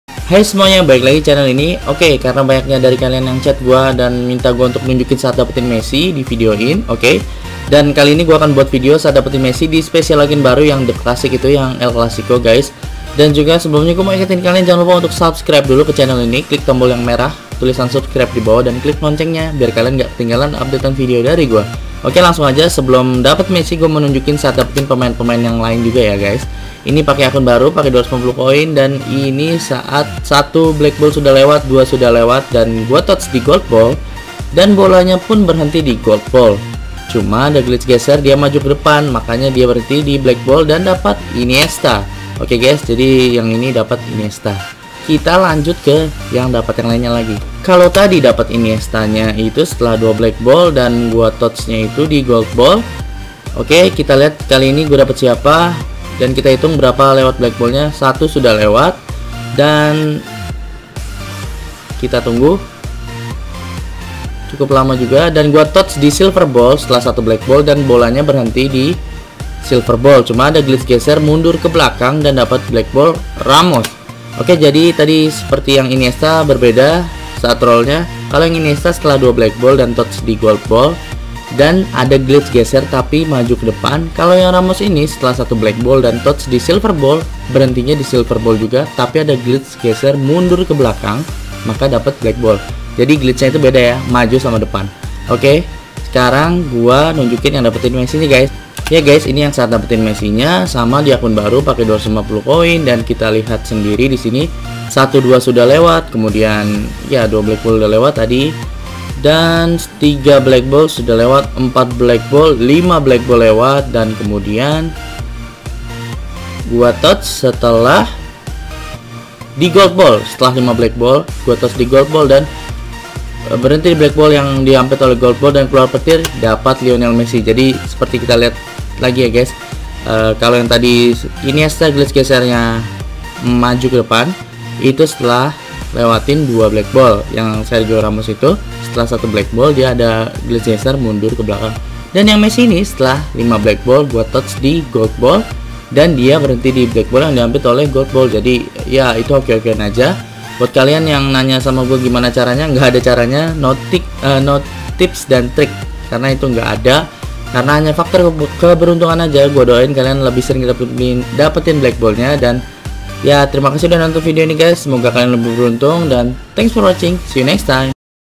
PROSES GW DAPAT BLACKBALL MESSI DARI SPESIAL AGENT THE CLASSIC !!!
Kali ini gue akan tunjukan saat Gue dapatkan BB Messi dari Spesial Agent terbaru yaitu THE CLASSIC ... Semoga kita semua bisa lebih beruntung dan bisa lebih sering mendapatkan blackball nya ya :)